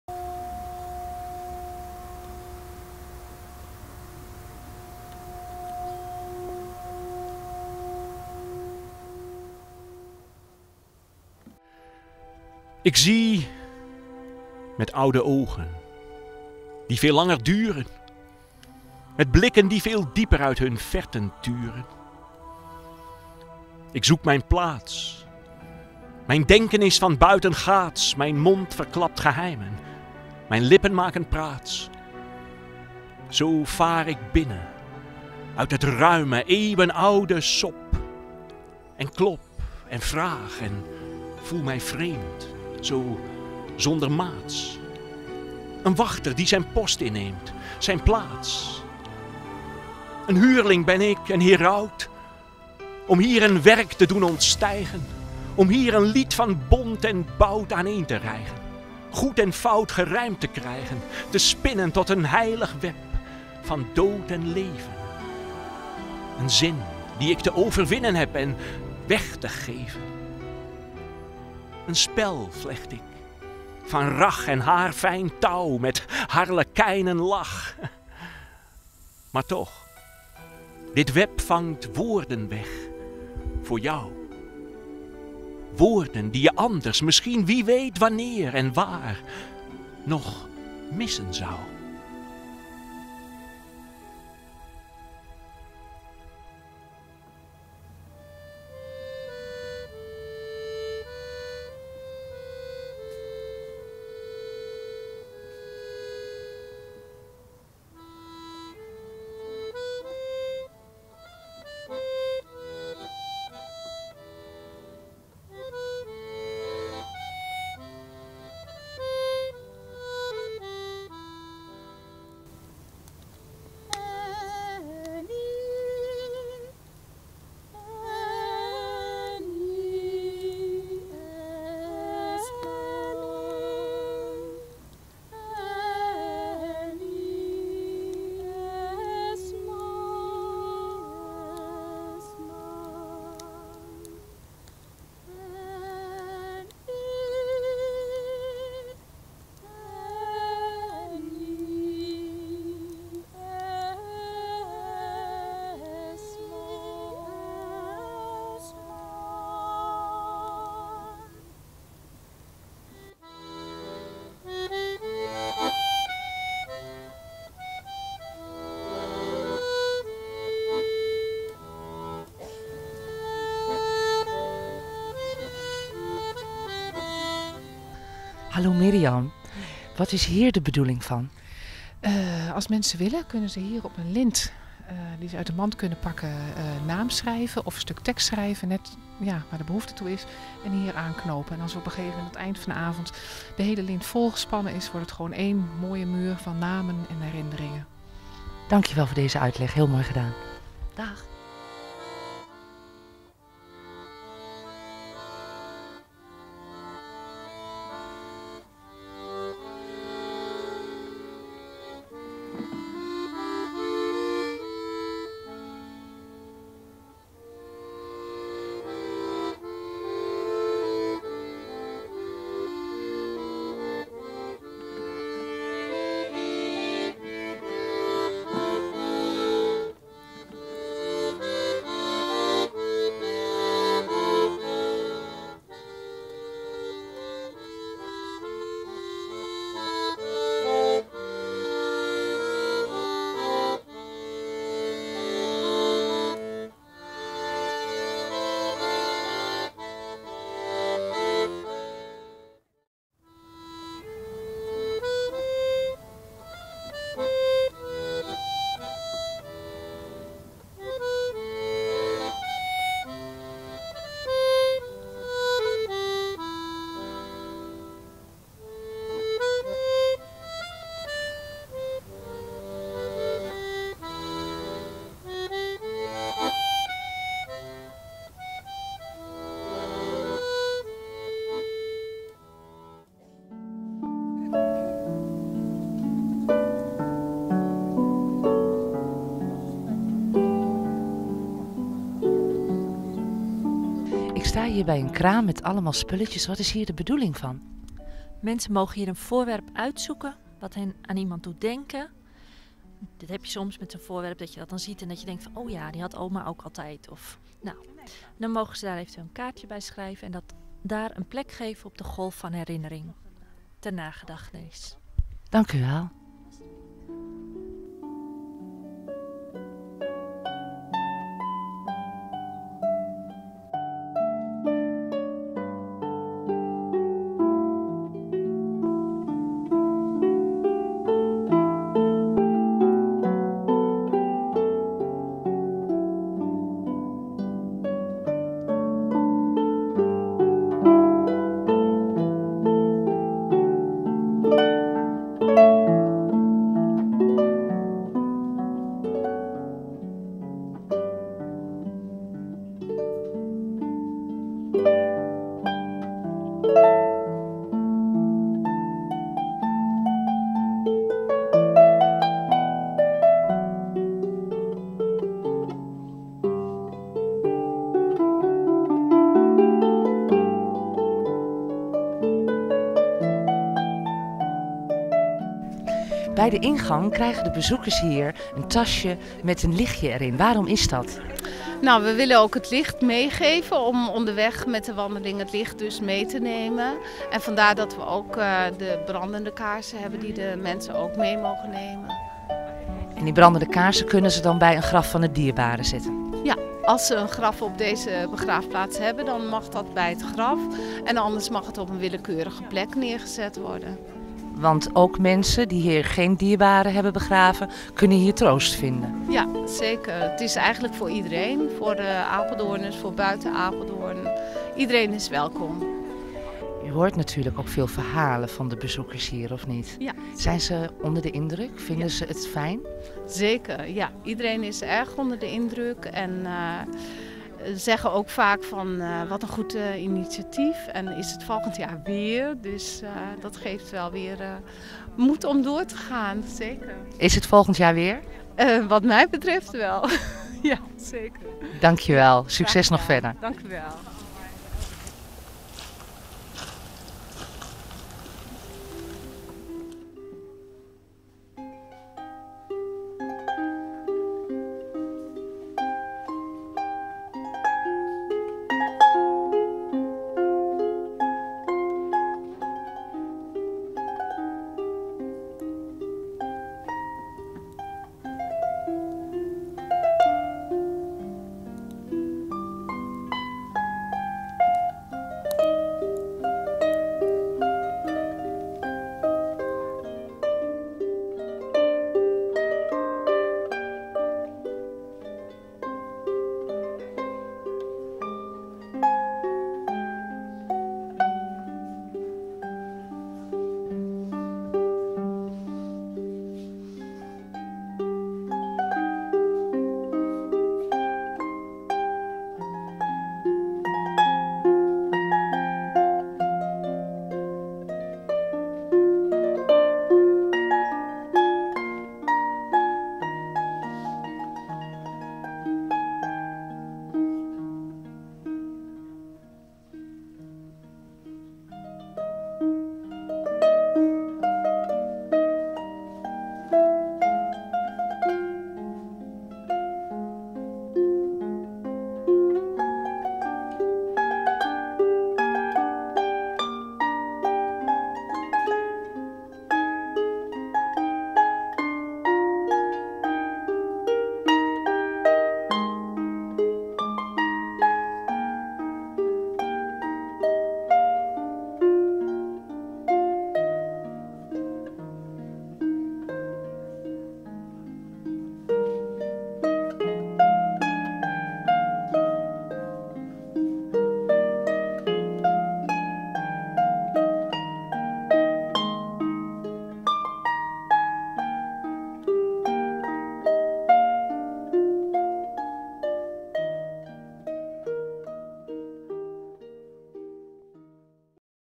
Laristho Cinema's Presenteert Allerzielen Alom 2009
Laristho Cinema's maakte een  korte impressie van: "De Doden niet verzwijgen, maar vieren om wie ze waren (Allerzielen Alom)

Gefilmd op dinsdagavond 4 november 2009. Op de begraafplaats aan de Soerenseweg .

Het initiatief kwam van Ida van Lee, die geassisteert werd door Alinde Vrolijk, Mirjan Jansen, Maria de Greef, en natuurlijk Gemeente Apeldoorn. De avond draaide om dankbaarheid te vieren voor wat er is geweest, en niet om het rouwen van wat er niet meer is. Allerzielen Alom was bedoeld voor iedereen die overleden familie, vrienden of bekenden op persoonlijke wijze wilden herdenken. Verschillende manieren van herdenken kwamen aan bod; Er was onder andere de mogelijkheid om op de Stilteplek een kaarsje op te steken, te luisteren naar de harpiste Annet Veen of de fluitmuziek van Harald Friskorn, Gebruiksvoorwerpen leggen als tastbare herinnering op de Golf van Herinnering, een bezoek te brengen aan het huis van bezinnig geoganiseerd door Robert La Boresa (woordkunstenaar), de mogelijkheid om een de naam van uw dierbare gezongen te laten worden door Alinde Vrolijk en Maria Kopcke, ook de mogelijkheid een bloembol te planten, om de cyclus van het leven te symboliseren. Alles kunt u bewonderen in de impressie van deze avond.